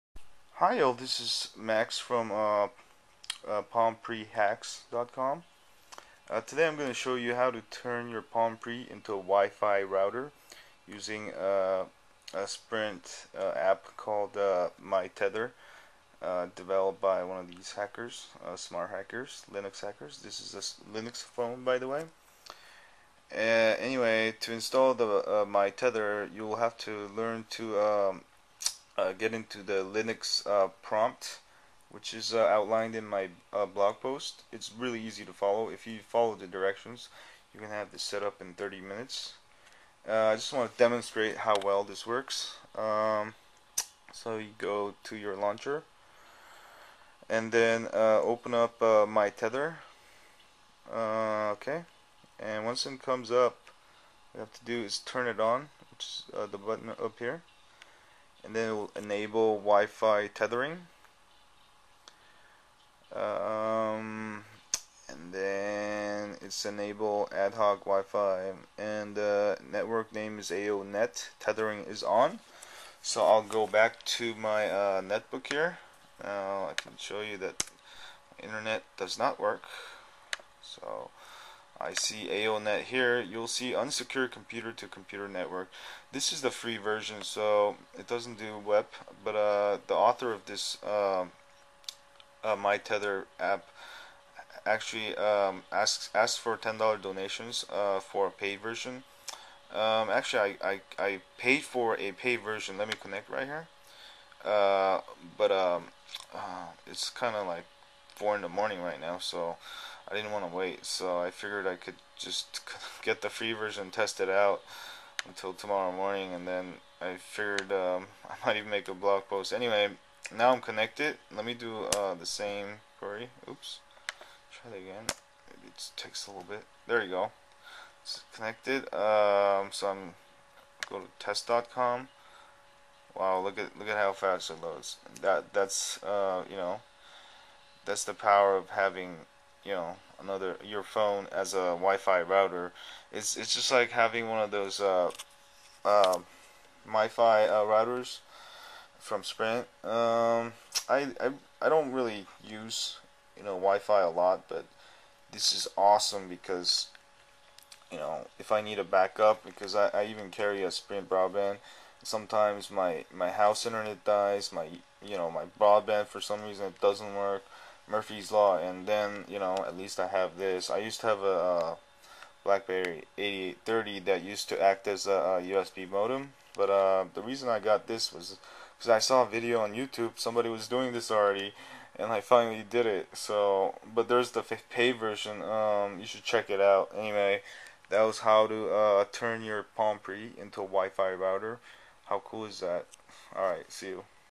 How to Hack (Tether) Palm Pre into WiFi Router!
Here's how to turn your Palm Pre into a WiFi router!